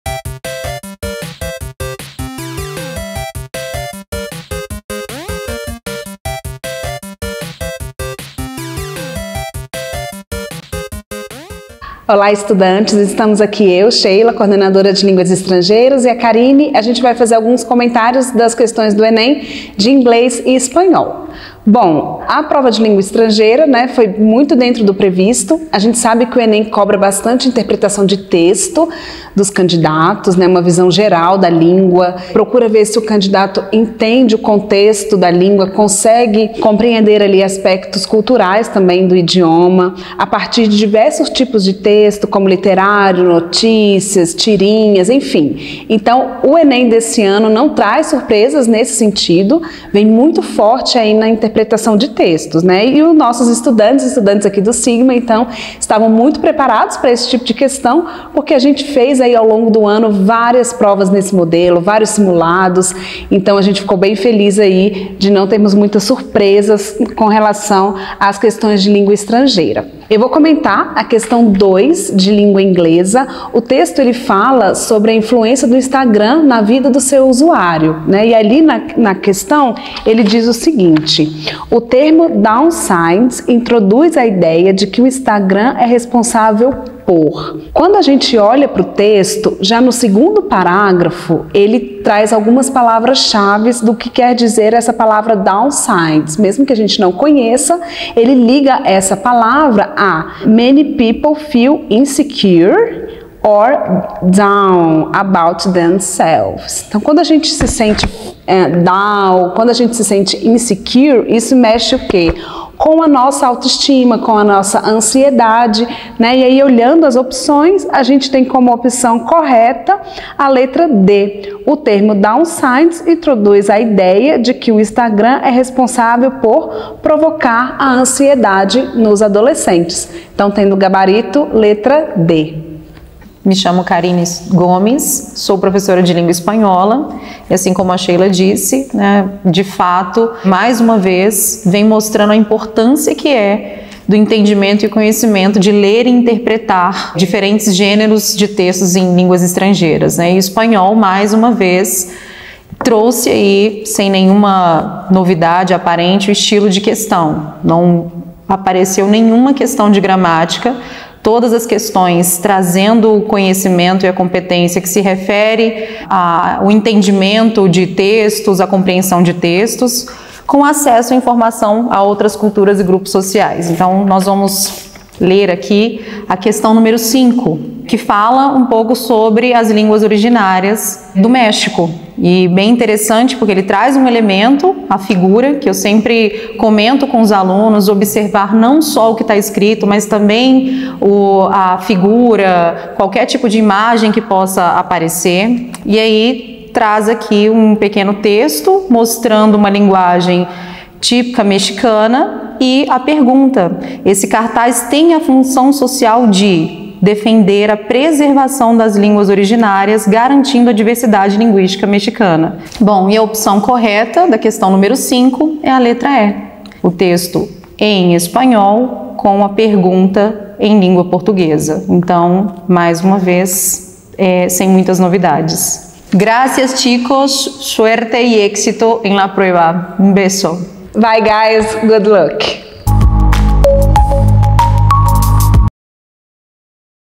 Resolução questões do enem2022 - Inglês / Espanhol
Neste ano, queremos que você gabarite todas as questões da prova, por isso confira a resolução das questões da prova do 1° dia do Enem 2022